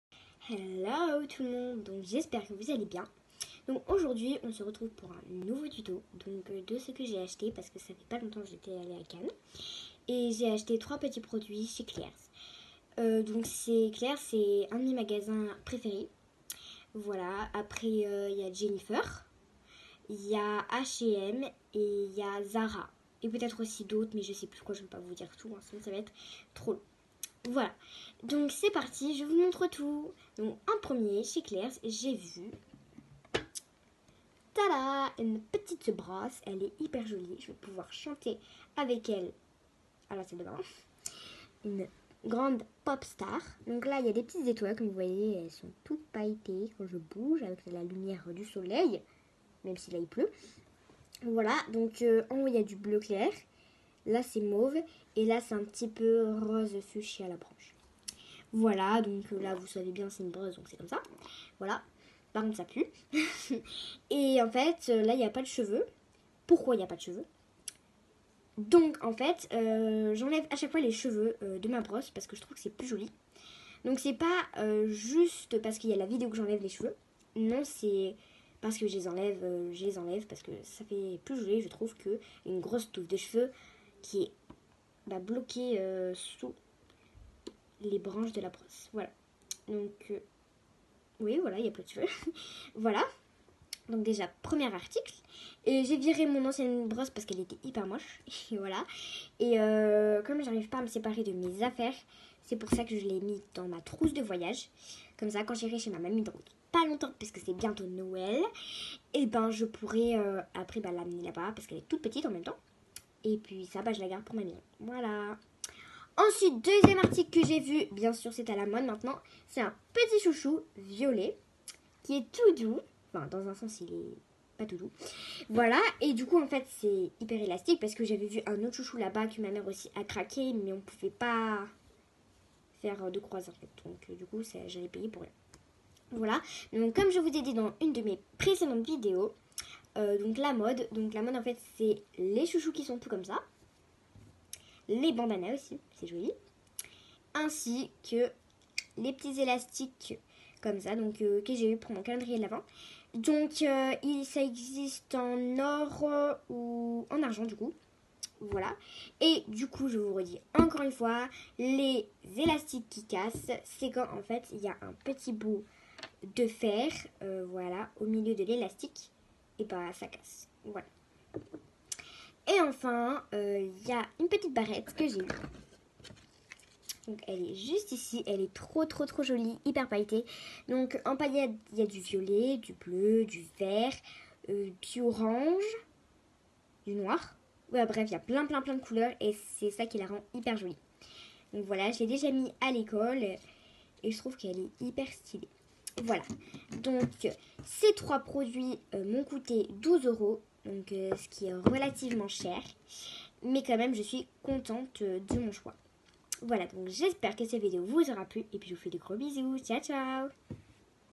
Mes achats chez claire's.
Je vous montre ce que j'ai acheté chez claire's